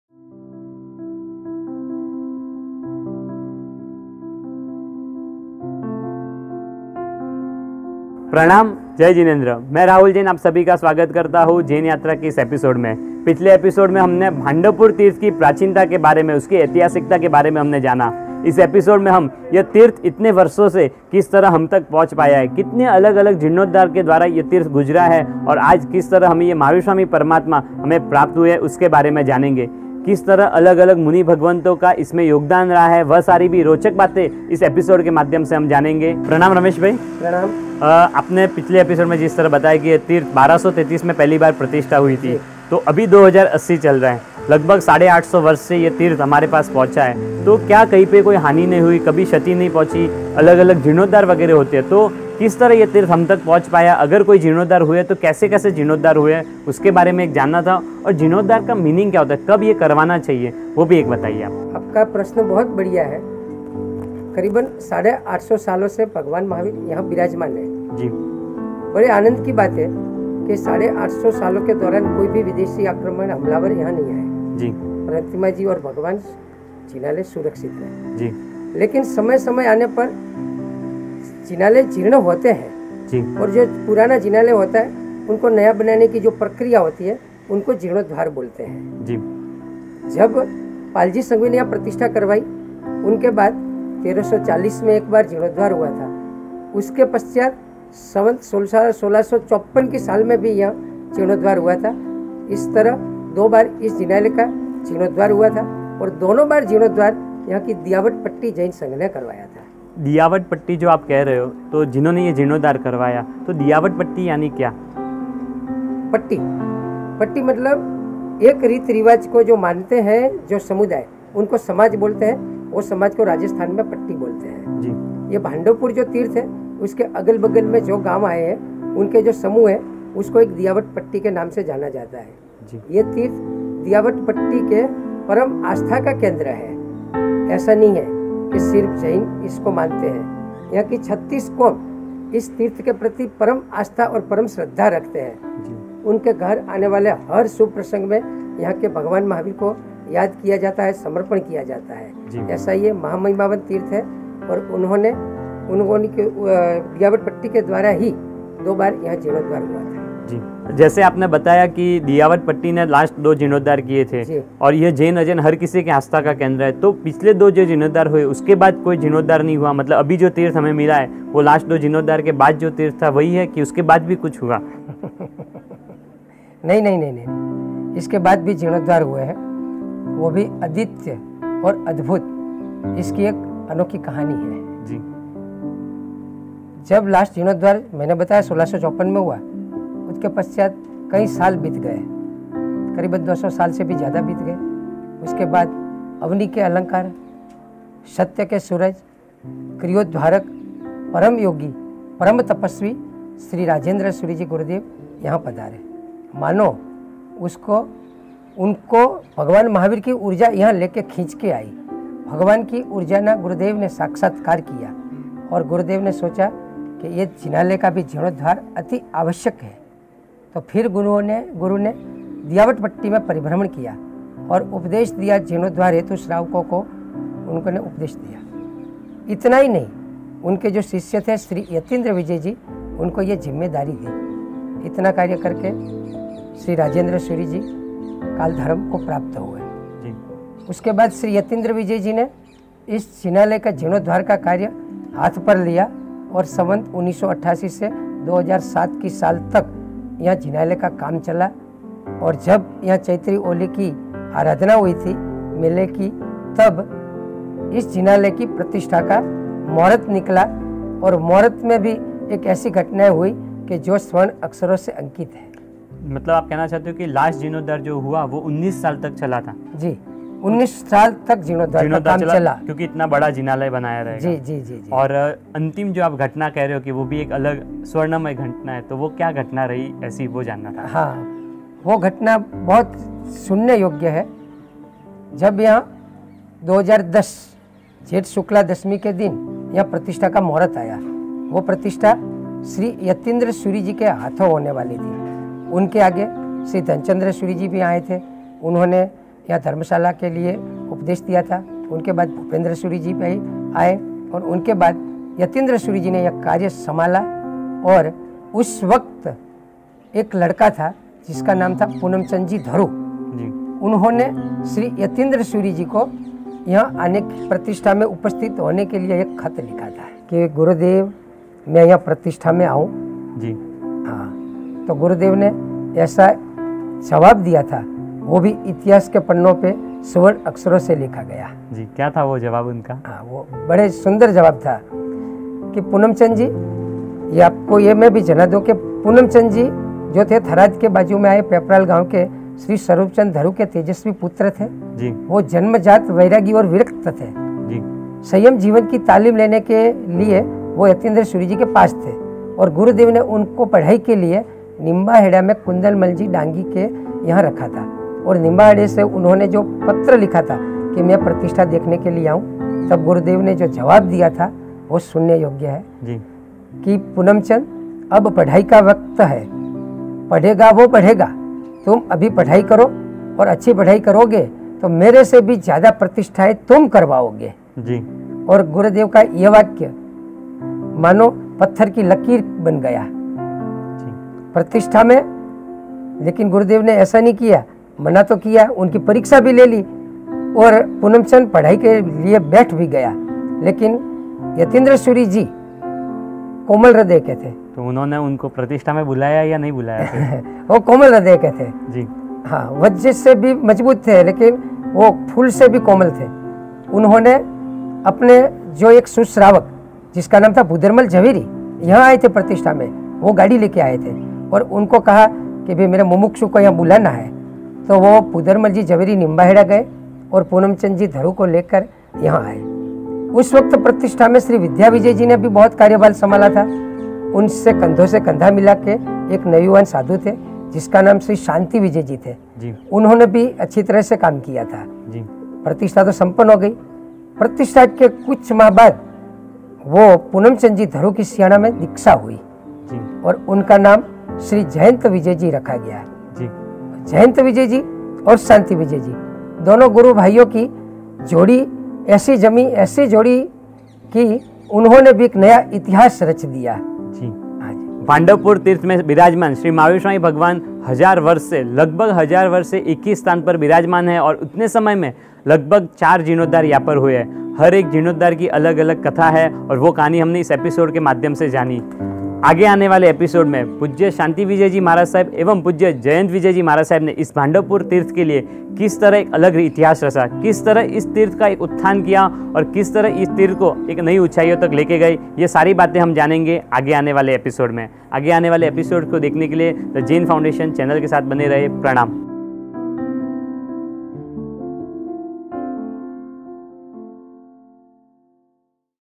श्री भांडवपुर तीर्थ का अनोखा इतिहास | Sri Bhandavpur Jain Tirth (Part - 2)
जिस तीर्थ की प्रतिष्ठा की बाते दुरदुर तक फेल रही है, जिस तीर्थ में देव गुरु और धर्म तीनो तत्वों की प्रतिष्ठा होनेवाली है, जिसकी प्रतिष्ठा को अब केवल एक माह बाकी है, एसे भांडवपुर तीर्थ की प्रतिष्ठा की बाते जान हम समर्पणम् परिवार के भाग्यशाली ओ को मिले, और उनसे सारी बात जानी। उन सभी बातो को जानने के लिए जुड़े रहे हमारे साथ...